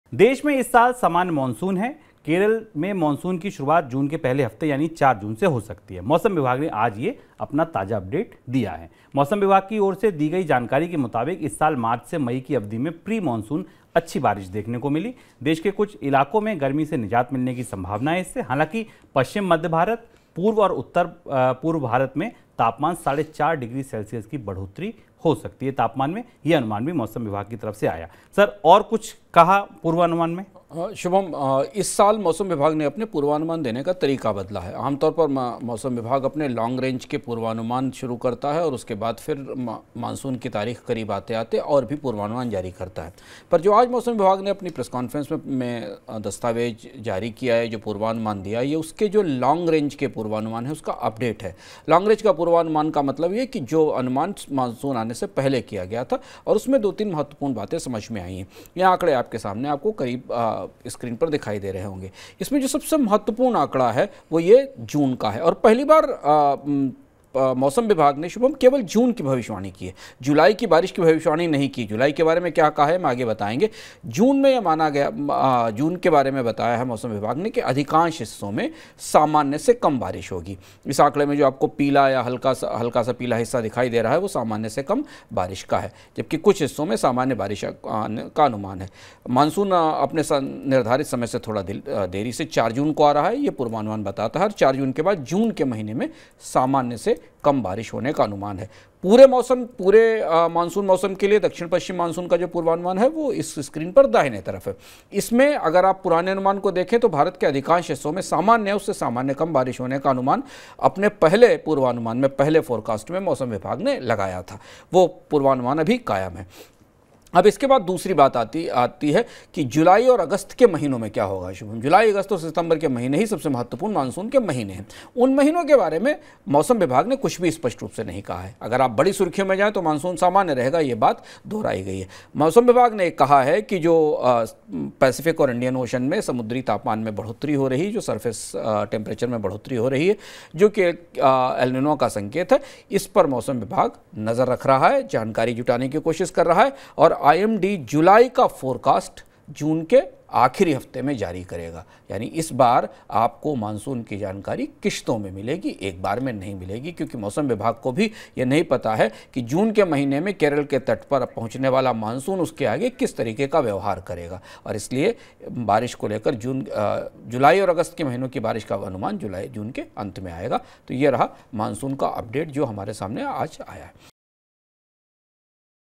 मानसून पर क्या है मौसम विभाग का ताजा अपडेट? Explained | Anshuman Tiwari | Money9
देश में इस साल सामान्य मॉनसून रहने का अनुमान है. केरल में मॉनसून की शुरुआत जून के पहले हफ्ते यानि 4 जून को हो सकती है. मौसम विभाग ने अपने ताजा अपडेट में यह जानकारी दी है. मौसम विभाग की ओर से दी गई जानकारी के मुताबिक इस साल मार्च से मई की अवधि में अच्छी प्री मॉनसून देखने को मिली है. देश के कुछ इलाकों में गर्मी से निजात मिलने की संभावना है. हालांकि पश्चिम मध्य भारत, पूर्व और उत्तर पूर्व भारत में तापमान में 4.5 डिग्री सेल्सियस की बढ़ोतरी होने का अनुमान है.